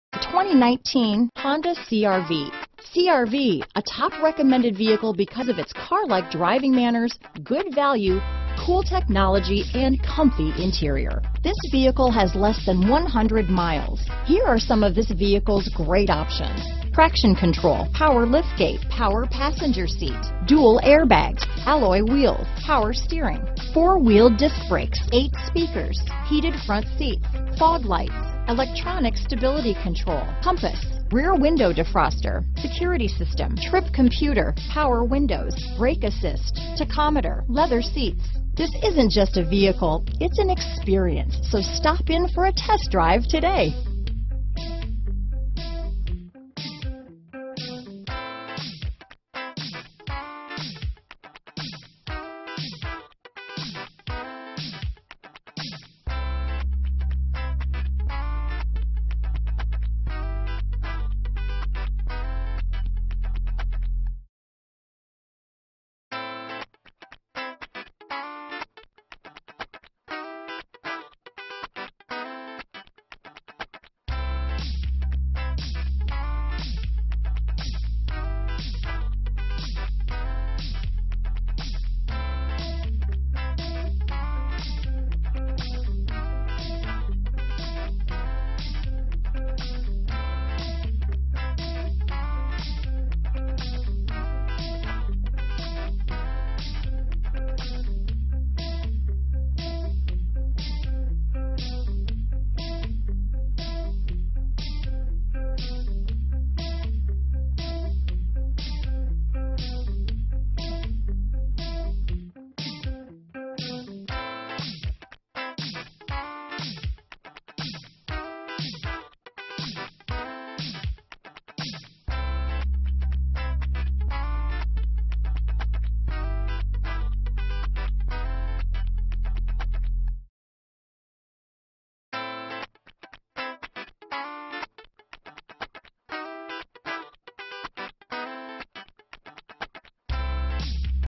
2019 Honda CR-V Columbia, Lexington, Irmo, West Columbia, Aiken, SC 4291209
Basque Red Pearl II N 2019 Honda CR-V available in Columbia, South Carolina at Honda of Columbia. Servicing the Lexington, Irmo, West Columbia, Aiken, SC area.

2019 Honda CR-V EX-L 28/34 City/Highway MPG 28/34 City/Highway MPG